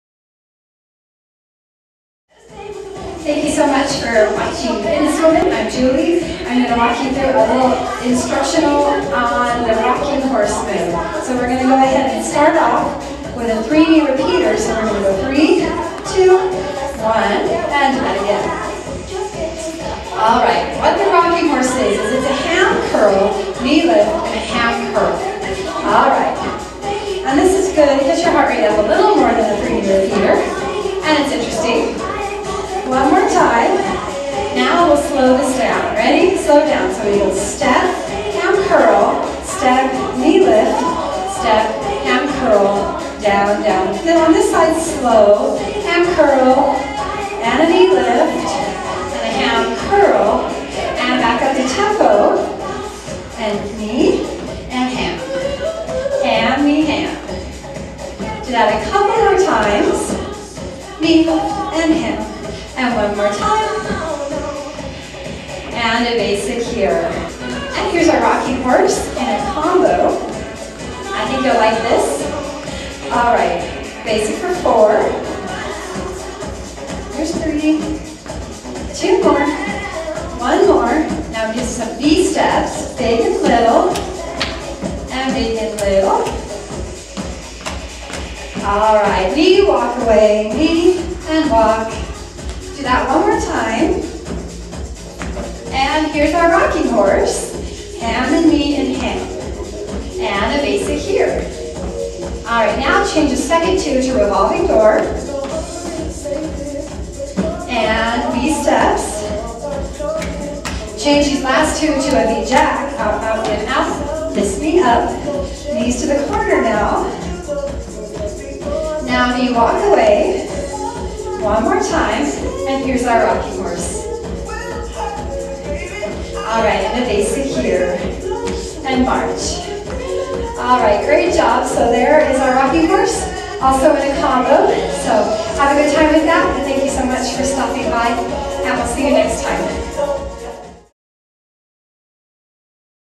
Free Aerobic Step Bench Workout Instructional Rocking Horse
Fitness Woman shows us the Step-by-Step instructions for the step bench move, The Rocking Horse, a fun replacement for the three knee repeater.  She finishes by putting it into a quick combo. Enjoy the move, and don't be afraid to get that healthy heart rate going!